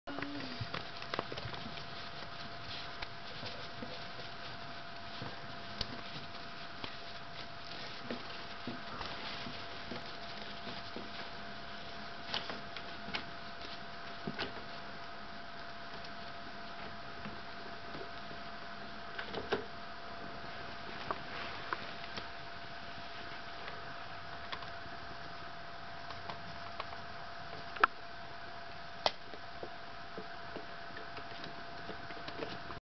My pet rat climbing up drawers!
This is a video of my very adventurous and energetic rat, Saffy, climbing her way up the side of a chest of drawers.